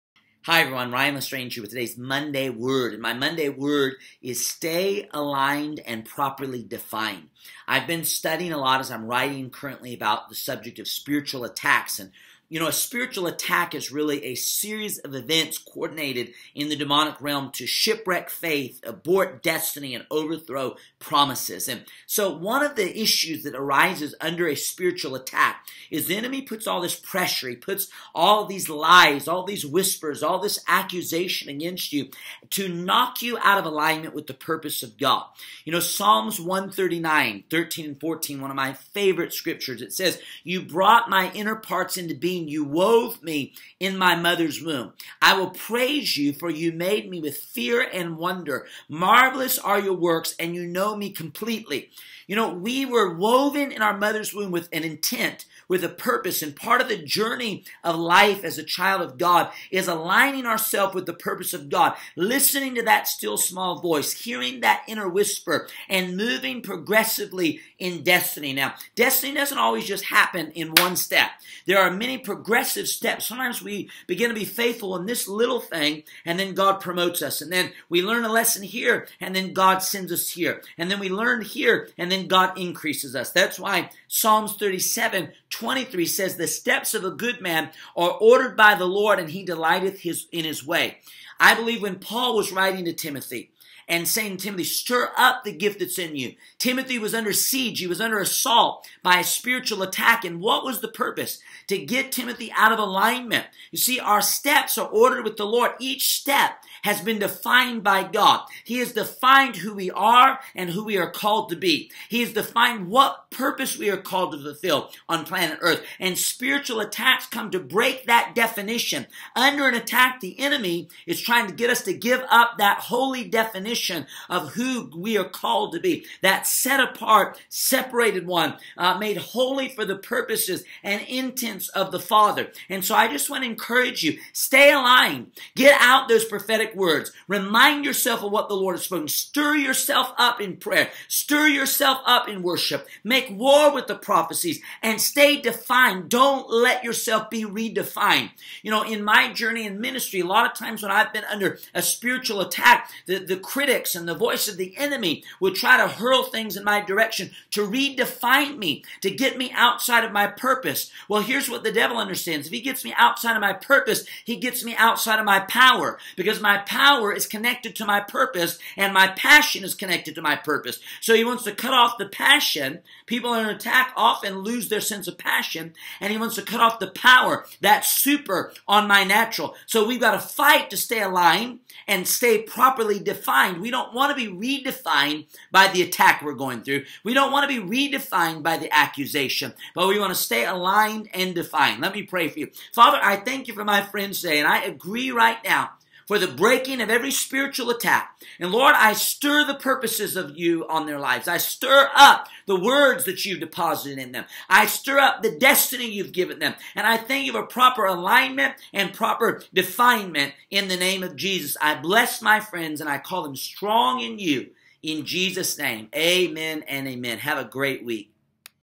MONDAY WORD-Stay aligned & properly defined.
When spiritual attacks arise one of the purposes is to remove the believer from destiny. In this Monday Word Ryan encourages the viewer to stay planted in destiny.